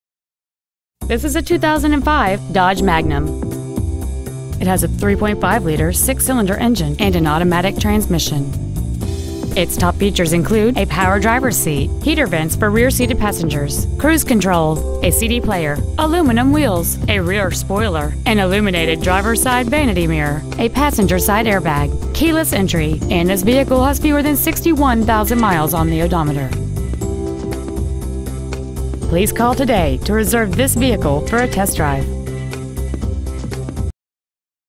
Preowned 2005 Dodge Magnum Worthington OH
We are proud to present this 2005 Dodge Magnum.

 Year: 2005
 Make: Dodge
 Model: Magnum
 Engine: 3.5L 6-Cylinder
 Trans.: automatic
 Exterior: Inferno Red Crystal Pearl
 Miles: 60,012
 Interior: Slate/Graystone

 Used 2005 Dodge Magnum Worthington OH